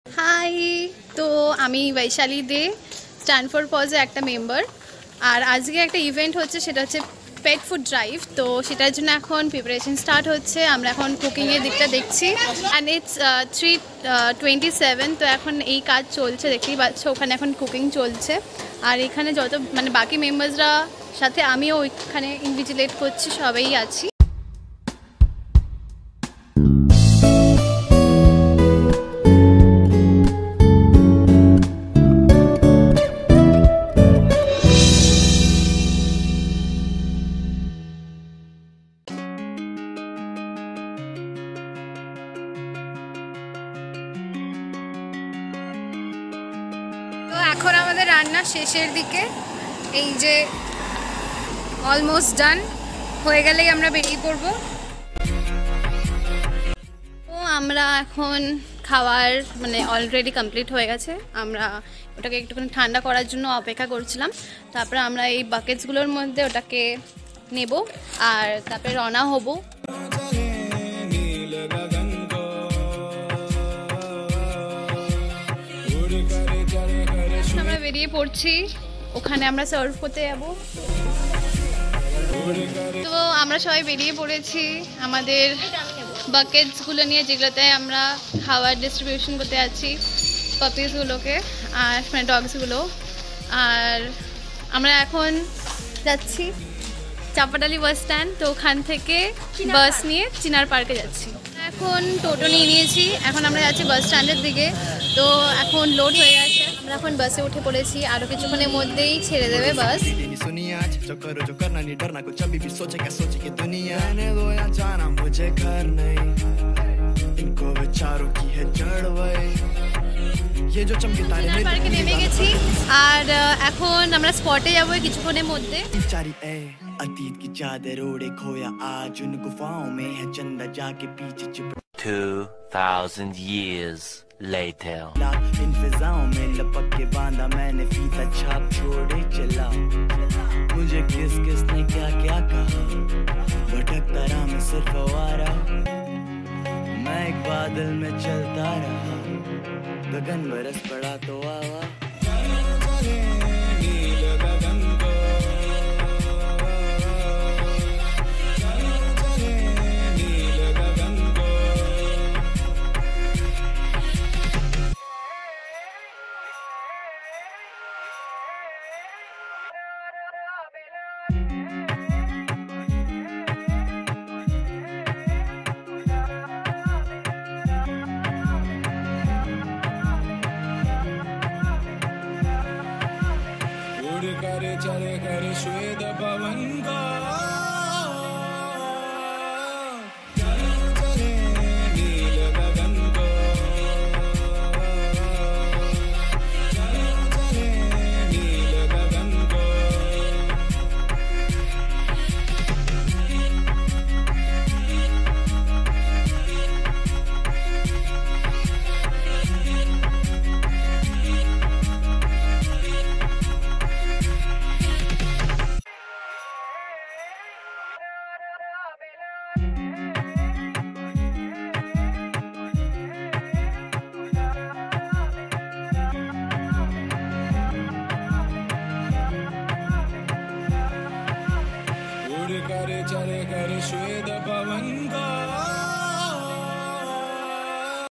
Vlog : 2 | Pet Food Drive | Day 1 | Eternal Bliss Official
This is the first day of our two week long Pet Food Drive for the strays. The day was filled with anticipation, hard work and a little bit of satisfaction. We are glad that we could feed these little angels and show them our love. We hope to bring up such exciting projects in the future as well and keep executing them.
Stand For Paws is the newest wing of ETERNAL BLISS working specifically on animal welfare. Please support us like you have always done.